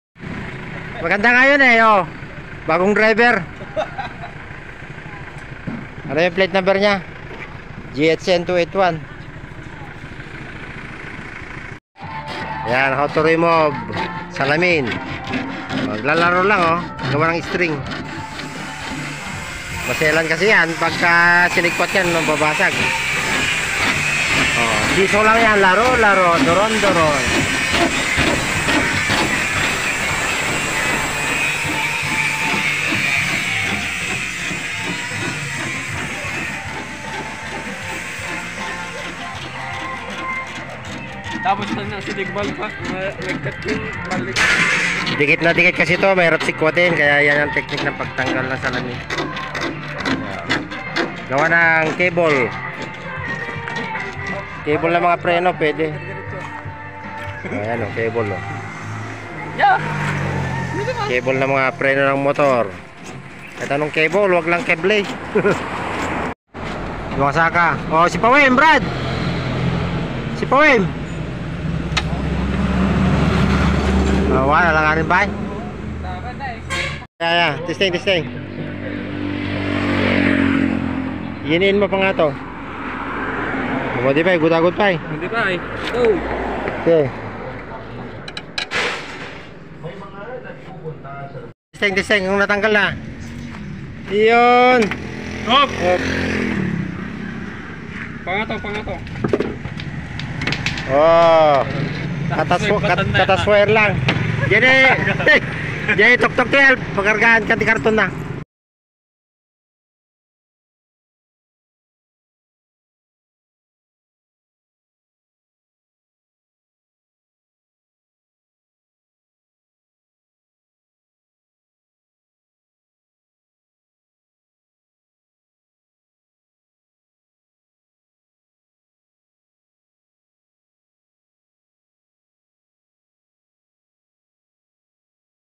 Mazda 1998 parts out ..
here's another towing and parts out guys . victim of minor accident..we check and start the engine first before we cut..all systems working well.. engine,Aircon ,power steering, automatic shifter are all good.. hope someone likes it.. thanks 👍